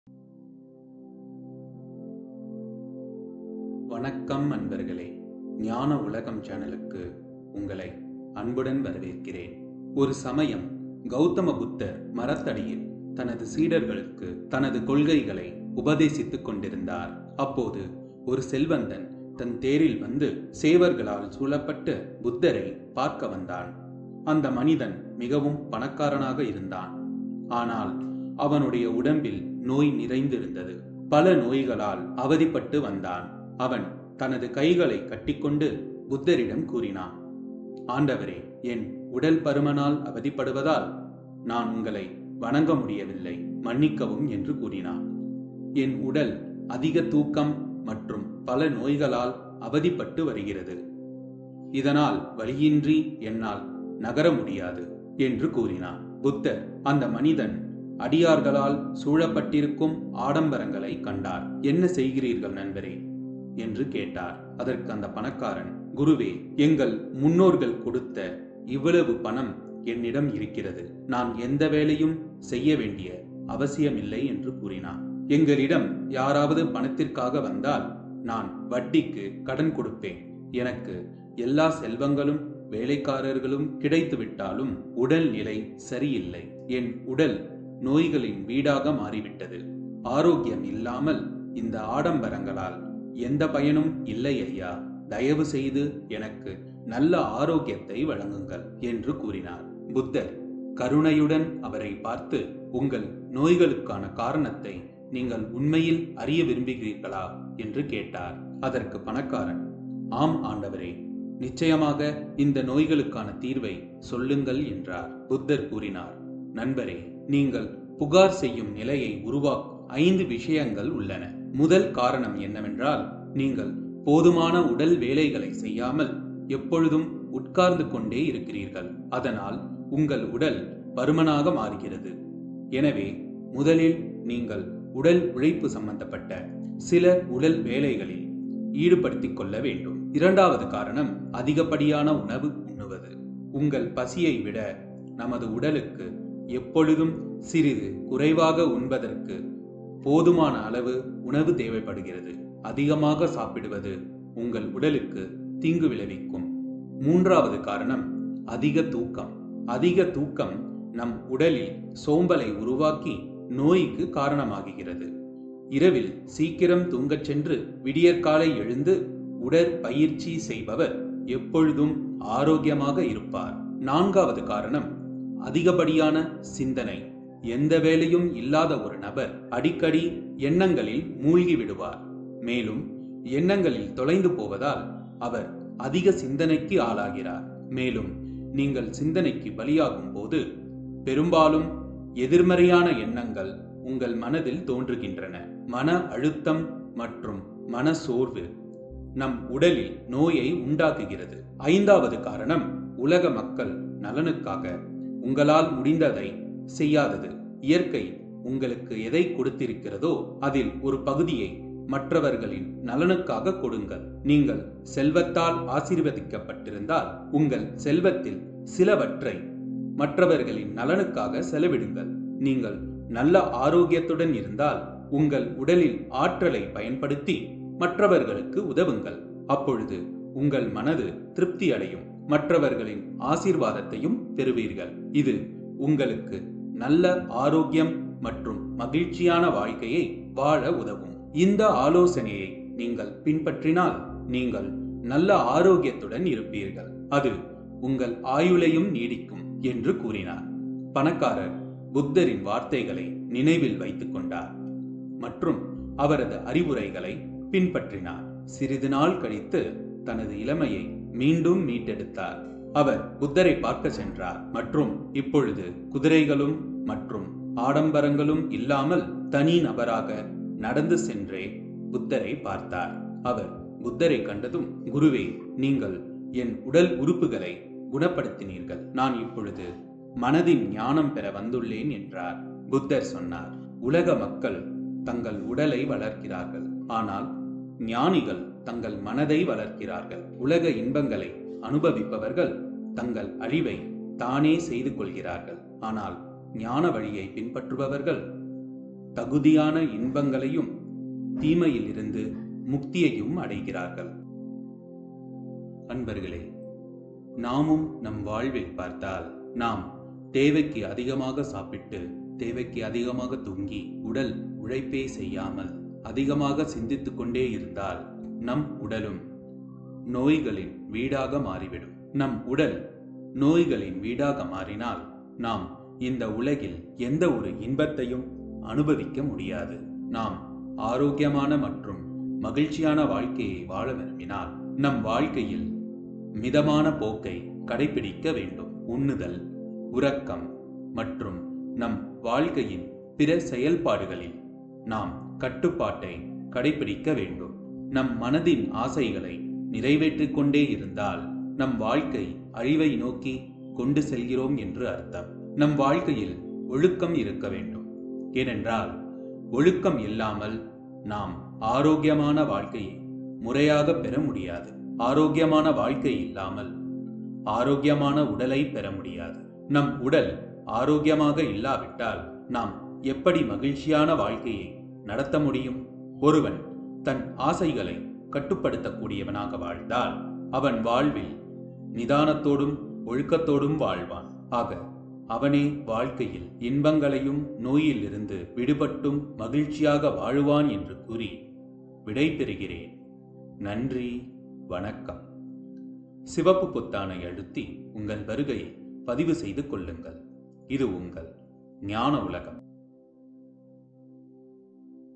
Five Ways to Keep Your Body and Mind Healthy | Buddha's teaching | Enlightenment World
In this video, I have shared the buddha's teaching of five ways to keep your body and mind healthy. We could see in our life that, so many people are lazy, due to that they give a home to diseases. this story help to understand and realize the importance of being active. Hope this story helps for those you need the most.

 healthy life
how to live without disease
how to overcome laziness